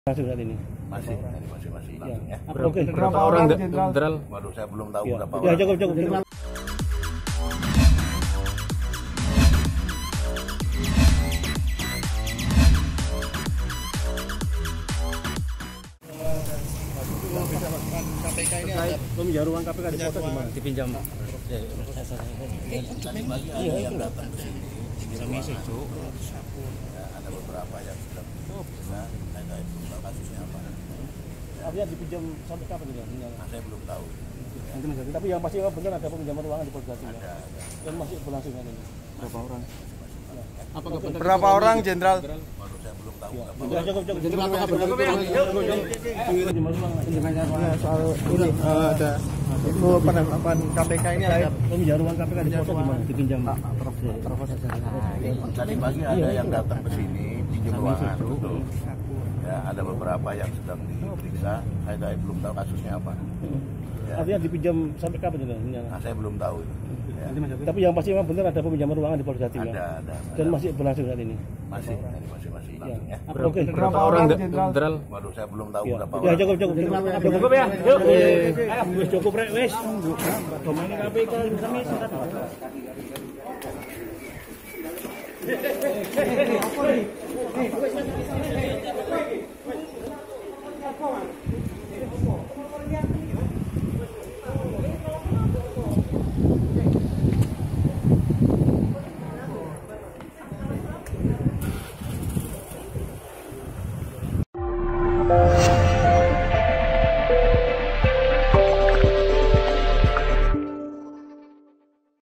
Penjelasan Kapolda Jatim terkait pemeriksaan Ketua Parpol yang tertangkap OTT
Laporan Wartawan TribunJatim.com, Syamsul Arifin

TRIBUNJATIM.COM, SURABAYA - Penjelasan Kapolda Jatim terkait pemeriksaan Ketua Parpol yang tertangkap OTT, Jumat (15/03/2019).